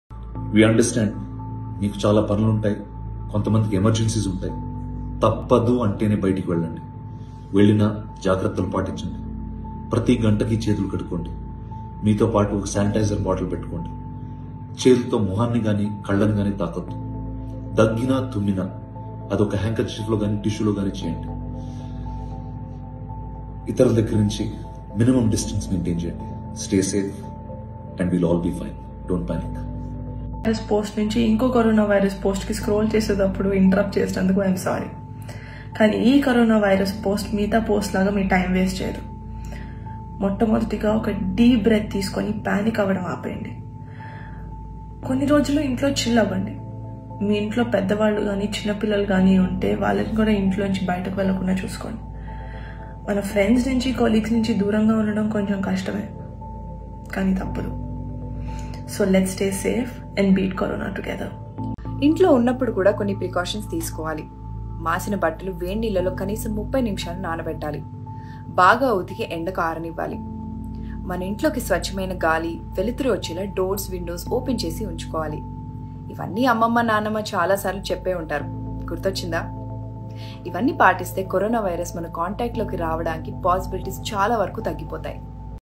Rana Daggubati & Niharika Message To Public On Present Situation | Stay Home Stay Safe | APTS Buzz
Welcome to APTS Buzz

Watch Here

Rana Daggubati & Niharika Message To Public On Present Situation

Latest Political News Updates

APTS Buzz